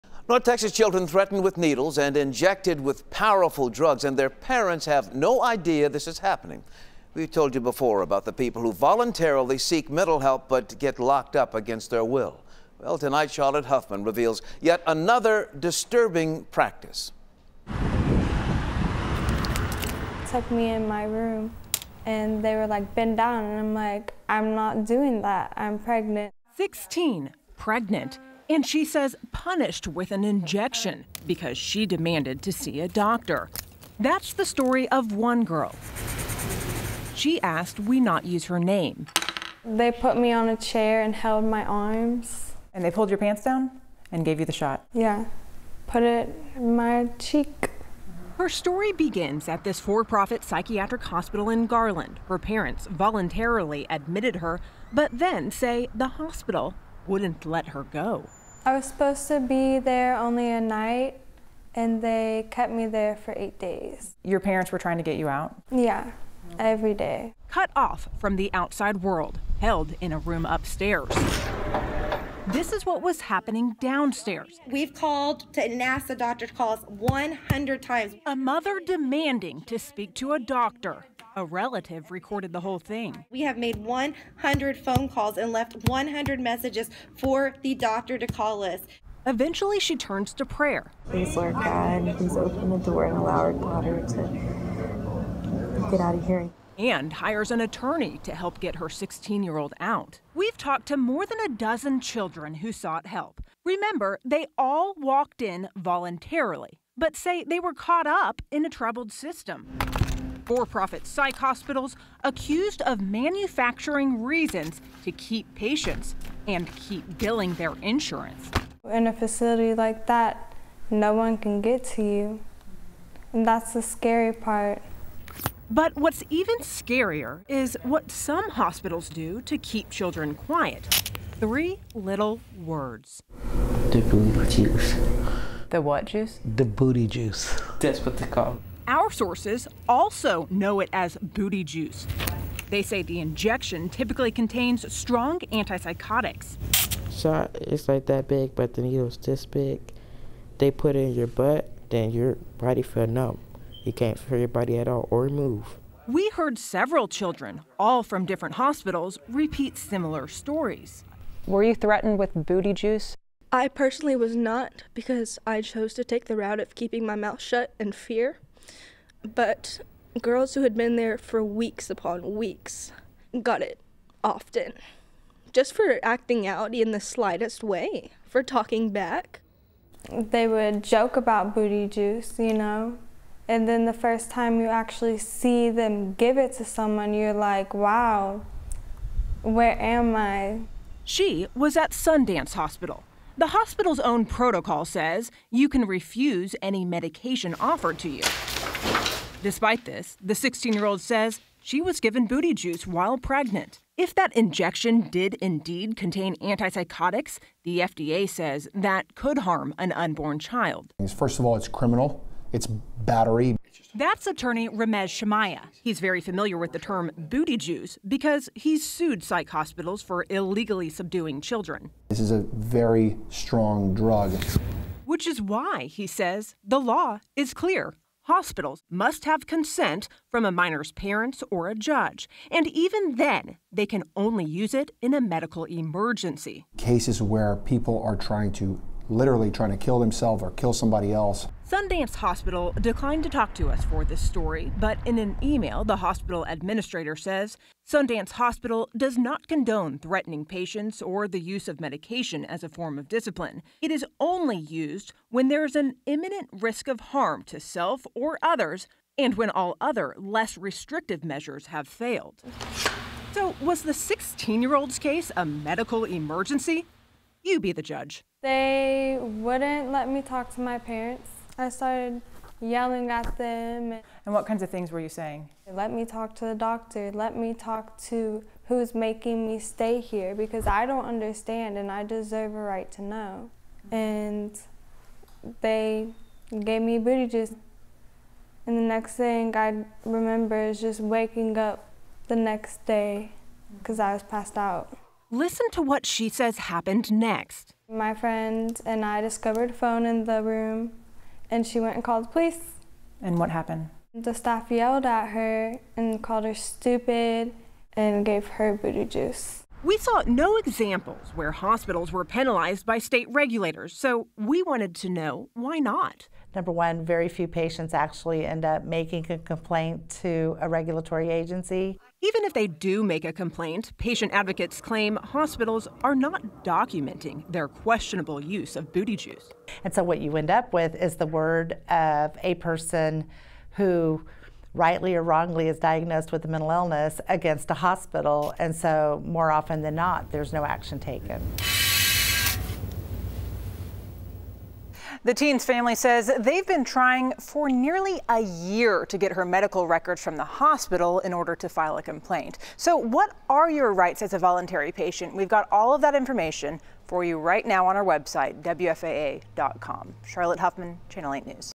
Patients say sedative known as booty juice injected against their will
A growing number of children and teenagers admitted voluntarily to North Texas for-profit psychiatric hospitals told WFAA that they've been injected with powerful sedative drugs without their parents’ knowledge.

The young patients and their parents said staffers at several psychiatric hospitals describe the practice as injecting patients with "booty juice." They claimed the injections often were administered in response to even minor disciplinary reasons.

One 16-year-old patient told WFAA that she was injected in her buttock with “booty juice” because she demanded to see a doctor.

“They took me in my room and they were like, 'Bend down,'" said the patient, who declined to be identified. “…They put me on a chair and held my arms…(They) put it in my cheek.”

She's among dozens of voluntary private psychiatric patients who've told WFAA they've been held against their will after seeking treatment.

Dallas Attorney Ramez Shamieh said he’s familiar with the term "booty juice." That’s because he has filed lawsuits against psychiatric hospitals for illegally subduing children.

WFAA became aware of the term booty juice in its series “Against Their Will.” During the interviews, various children and teenagers admitted voluntarily to several psychiatric hospitals said they received or were threatened by hospital staffers with booty juice injections.